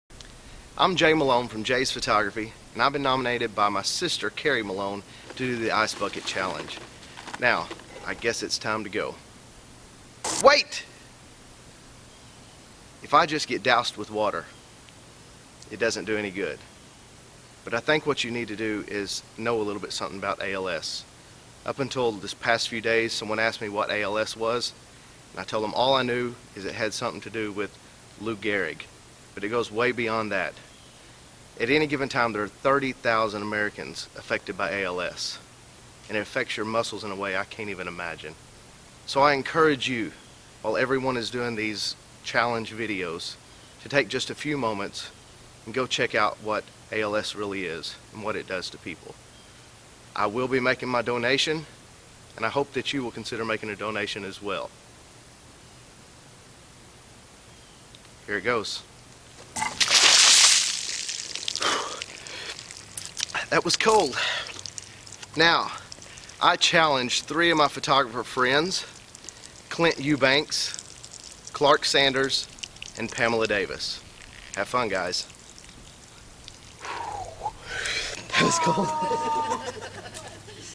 Jay Malone Ice Bucket Challenge - Water freezes in mid air!
Making water freeze in mid-air!

I was challenged by my sister, and I challenged 3 of my fellow photographers!  But more importantly, I hope that I made someone else aware, that they too need to research ALS.